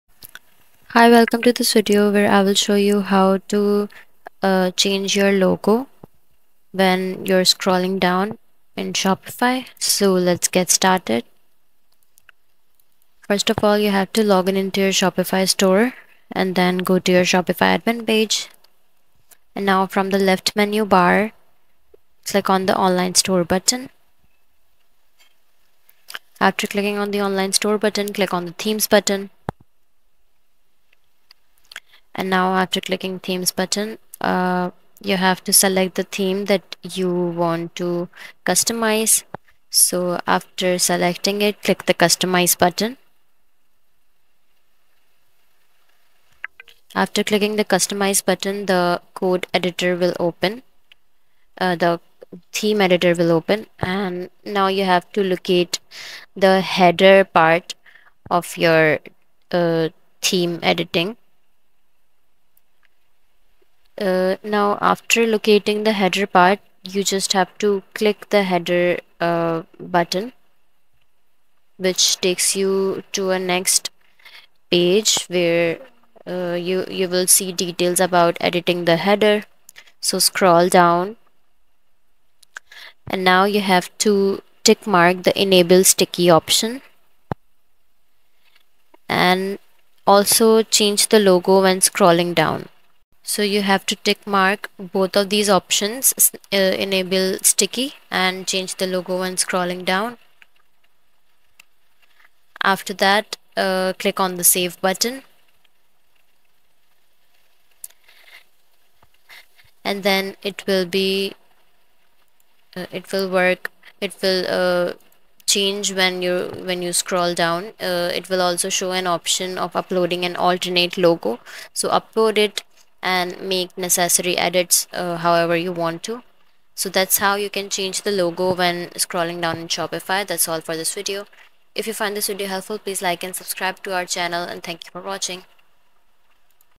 How To Change Logo When Scrolling Down Shopify (Step-By-Step Guide)
In this video tutorial we will walk you through step by step how to change logo when scrolling down Shopify? Are you looking to add a touch of dynamism and creativity to your Shopify store? In this step-by-step tutorial, we'll show you how to change your website's logo dynamically as users scroll down the page. This eye-catching effect can make your brand stand out and leave a lasting impression on your visitors.

How to change logo when scrolling down Shopify

Changing the logo while scrolling down the page can be a great way to showcase different aspects of your brand or add seasonal elements to your store. The best part? You don't need to be a coding expert to achieve this! We'll guide you through the entire process using simple and easy-to-understand instructions.

In this Shopify tutorial, you'll learn:

Preparing Your Logos: We'll start by creating the different logo variations you want to use on your site. Whether it's a color change, seasonal design, or special promotion, we'll help you create the perfect logos.

Uploading Logos to Shopify: Learn how to upload your custom logos to the Shopify theme editor. We'll make sure you're ready to implement the scrolling effect seamlessly.

Installing the Necessary Code: Don't worry about complex coding! We'll provide you with a ready-to-use code snippet that you can simply copy and paste into your Shopify theme.

Customizing the Scrolling Behavior: Want the logo to change size, color, or fade in and out? We'll show you how to customize the scrolling behavior to match your brand's aesthetics.

Testing and Troubleshooting: It's essential to test the changes before making them live. We'll guide you through testing the scrolling effect and help troubleshoot any issues you may encounter.

With our guidance, you'll be able to create a captivating and interactive browsing experience for your customers, enhancing their engagement and encouraging repeat visits.

Level: Beginner to Intermediate

Time Needed: Approximately 30 minutes

Prerequisites: Basic familiarity with Shopify's theme editor (no coding experience required).

Don't miss this opportunity to give your Shopify store a touch of professionalism and creativity! Join us now and transform your website with this engaging scrolling logo effect.